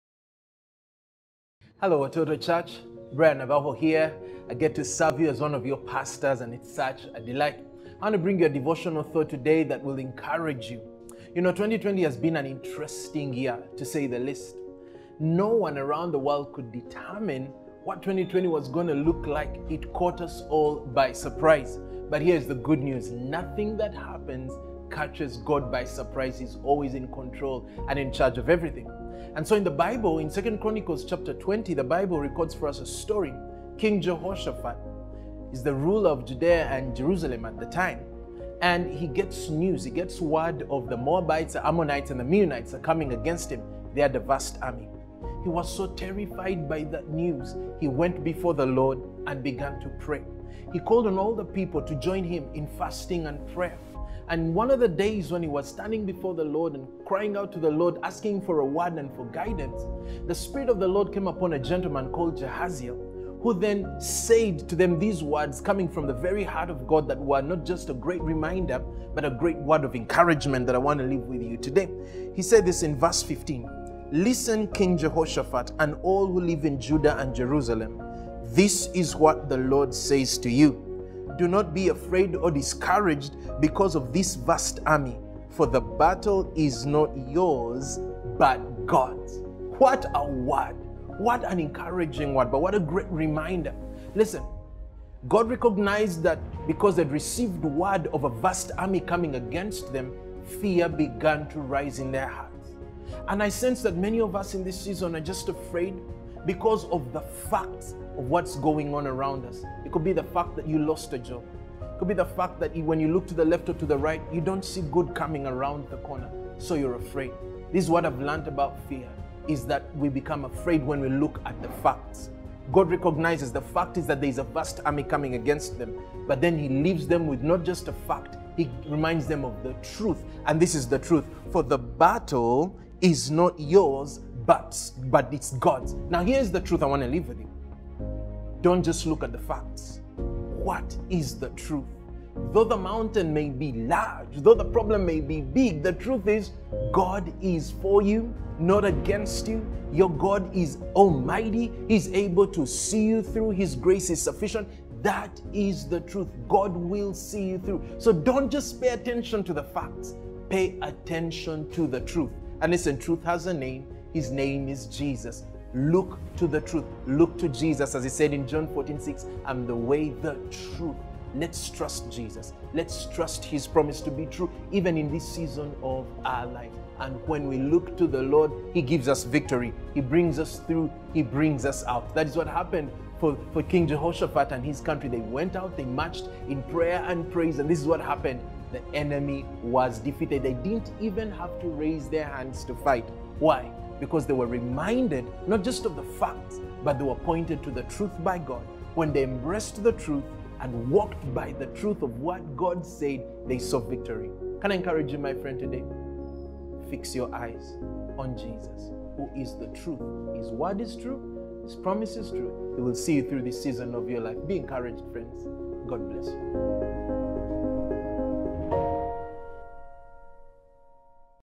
Today's Devotion by Ps Brian
Do not be afraid or discouraged by all the difficulties in your life for the battle is not yours, but God’s. The facts you see before you may be alarming, but look to the Truth - Jesus. He is your victory.